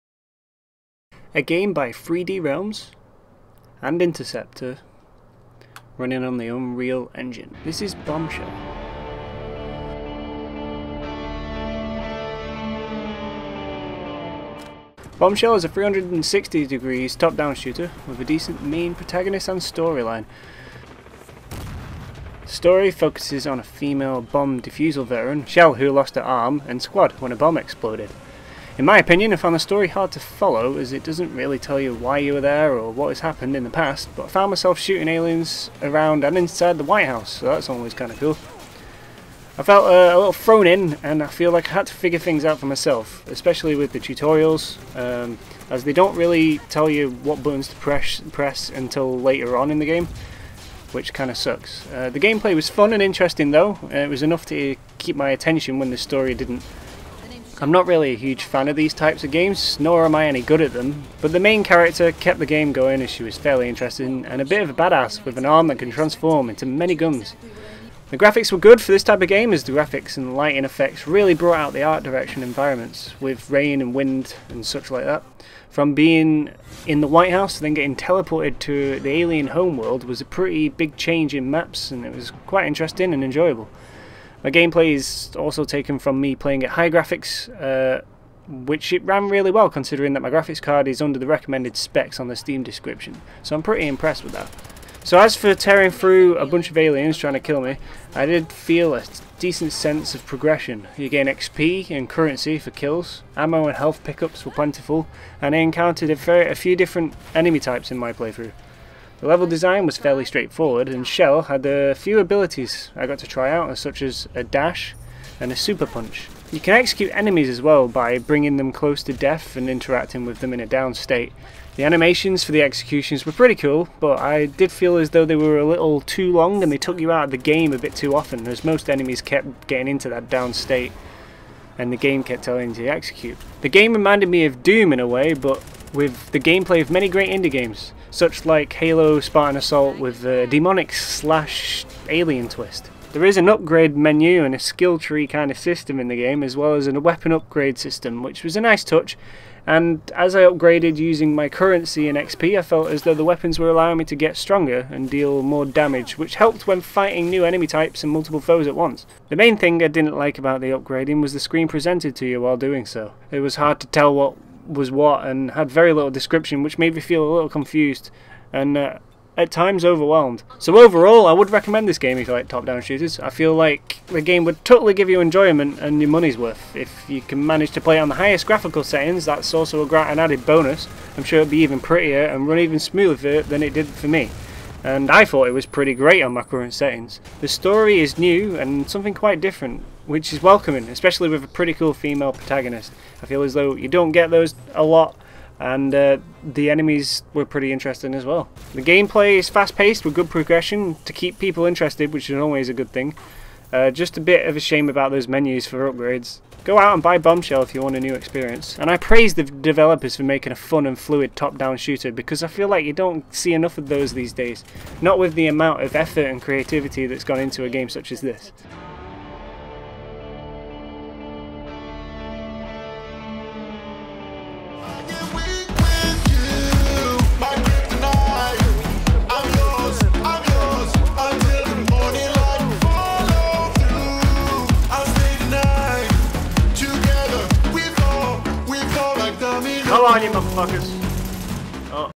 Bombshell | KTA Review
My very first Review, Hopefully it let's you know my opinions and gives you enough information on whether to buy the game or not!

Please leave some feedback and you could win a Destiny poster!